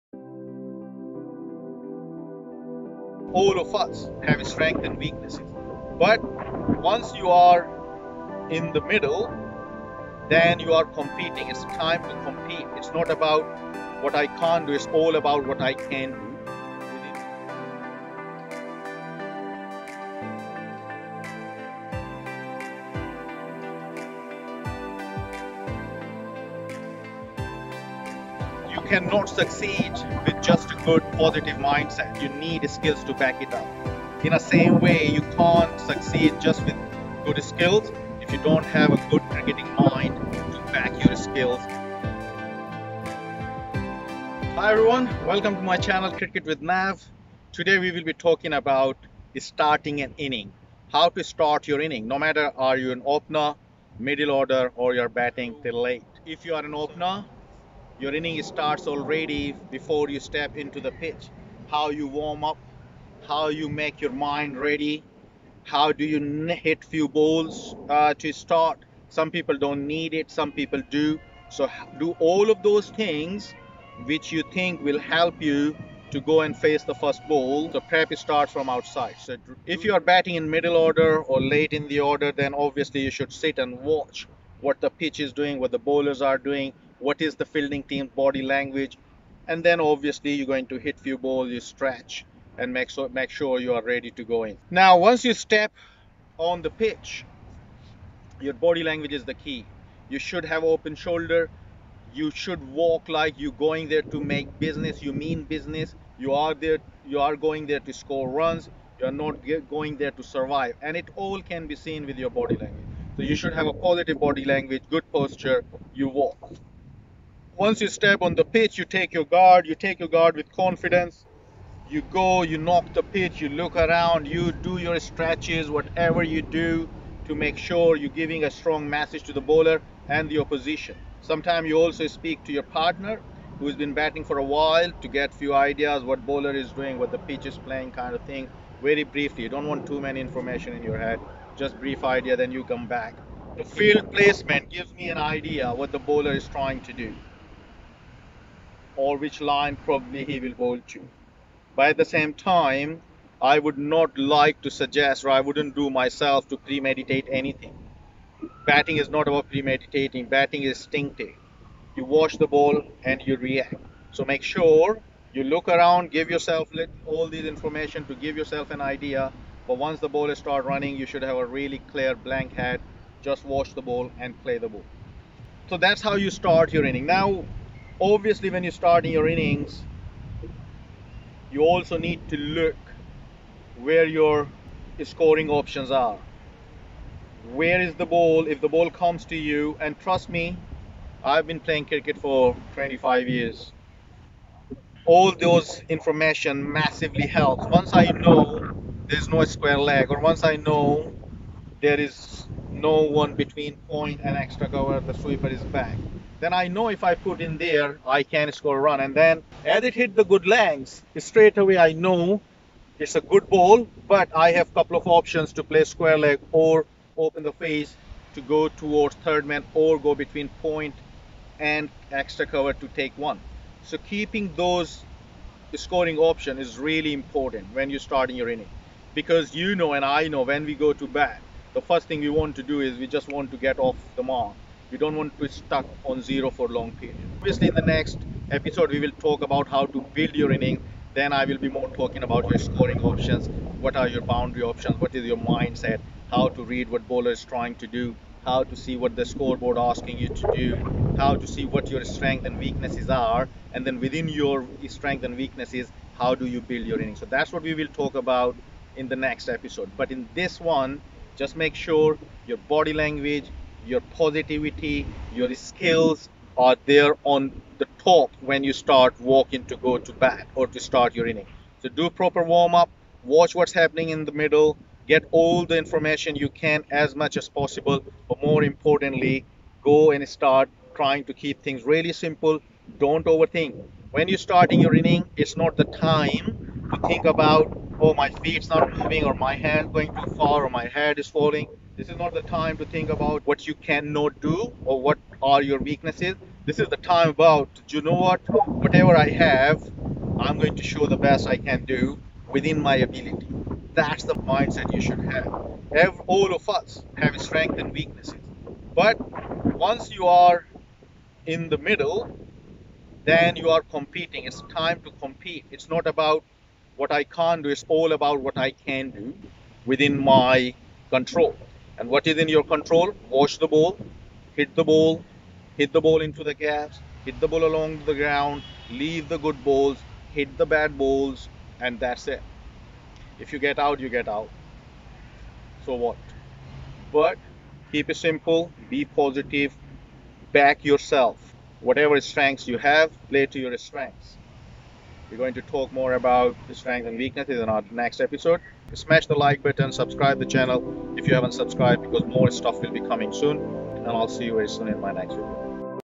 PROCESS TO START YOUR INNING: BATTING DRILLS
MOST IMPORTANT TIME FOR A BATTER IS TO START AN INNING. IN THIS VIDEO YOU WILL LEARN WHAT IS THE BEST WAY TO START AN INNING.